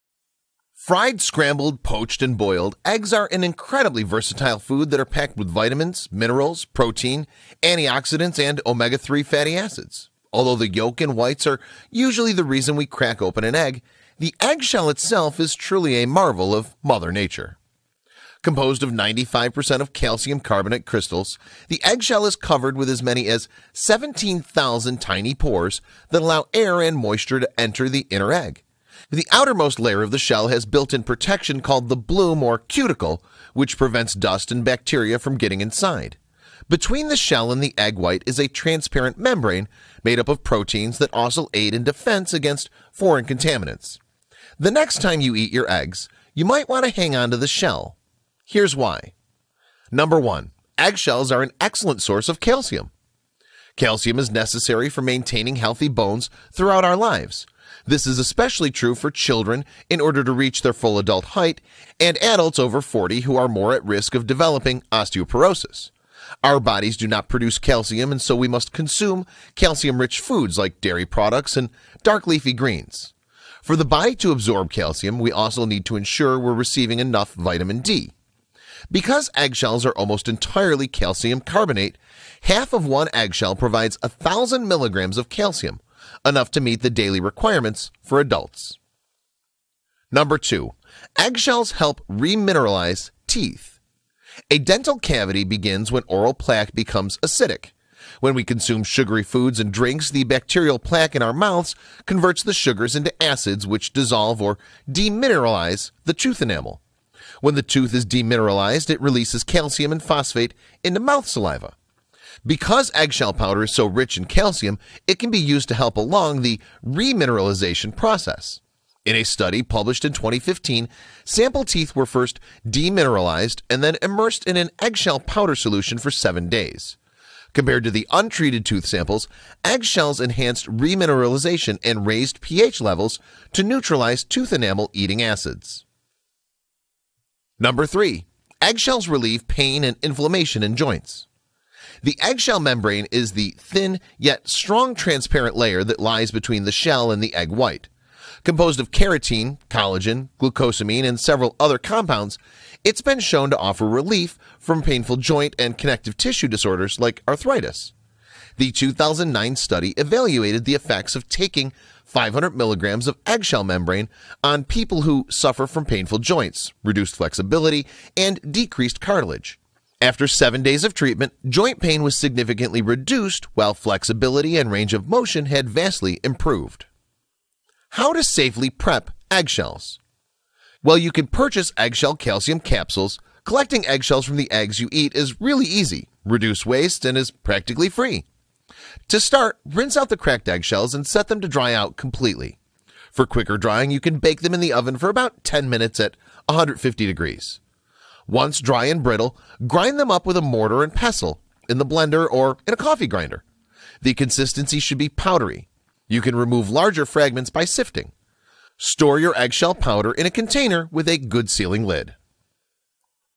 Surprising Reasons You should Eat Eggshells
Fried, scrambled, poached, and boiled, eggs are an incredibly versatile food that are packed with vitamins, minerals, protein, antioxidants, and omega-3 fatty acids.
Although the yolk and whites are usually the reason we crack open an egg, the eggshell itself is truly a marvel of Mother Nature.
Composed of 95% of calcium carbonate crystals, the eggshell is covered with as many as 17,000 tiny pores that allow air and moisture to enter the inner egg. The outermost layer of the shell has built in protection, called the bloom or cuticle, which prevents dust and bacteria from getting inside. Between the shell and the egg white is a transparent membrane, made up of proteins that also aid in defense against foreign contaminants.
The next time you eat eggs, you might want to hang on to the shell. Here’s why.

1.Eggshells are an Excellent Source of Calcium
Calcium is necessary for maintaining healthy bones throughout our lives. This is especially true for children in order to reach their full adult height and adults over 40 who are more at risk of developing osteoporosis. Our bodies do not produce calcium and so we must consume calcium rich foods like dairy products and dark leafy greens. For the body to absorb calcium, we also need to ensure we are receiving enough vitamin D.
Because eggshells are almost entirely calcium carbonate, half of one eggshell provides 1,000 mg of calcium – enough to meet the daily requirements for adults.
2.Eggshells Help Re-mineralize Teeth
A dental cavity begins when oral plaque becomes acidic. When we consume sugary foods and drinks, the bacterial plaque in our mouths converts the sugars into acids which dissolve (or demineralize) the tooth enamel.
When the tooth is demineralized, it releases calcium and phosphate into mouth saliva. Because eggshell powder is so rich in calcium, it can be used to help along the remineralization process. In a study published in 2015, sample teeth were first demineralized and then immersed in an eggshell powder solution for seven days. Compared to the untreated tooth samples, eggshells enhanced remineralization and raised pH levels to neutralize tooth enamel eating acids.

3.Eggshells Relieve Pain & Inflammation in Joint
The eggshell membrane is the thin yet strong transparent layer that lies between the shell and the egg white. Composed of keratin, collagen, glucosamine, and several other compounds, it has been shown to offer relief from painful joint and connective tissue disorders like arthritis.
The 2009 study evaluated the effects of taking 500-mg of eggshell membrane on people who suffer from painful joints, reduced flexibility, and decreased cartilage. After seven days of treatment, joint pain was significantly reduced while flexibility and range of motion had vastly improved.
How to Safely Prep Eggshells?
While you can purchase eggshell calcium capsules, collecting eggshells from the eggs you eat is really easy, reduces waste, and is practically free.
To start, rinse out cracked eggshells and set them out to dry completely. For quicker drying, you can bake them in the oven for about 10 minutes at 150°F. Once dry and brittle, grind them up with a mortar and pestle, in the blender, or a coffee grinder. The consistency should be powdery; you can remove larger fragments by sifting. Store your eggshell powder in a container with a good sealing lid.

Pinterest.com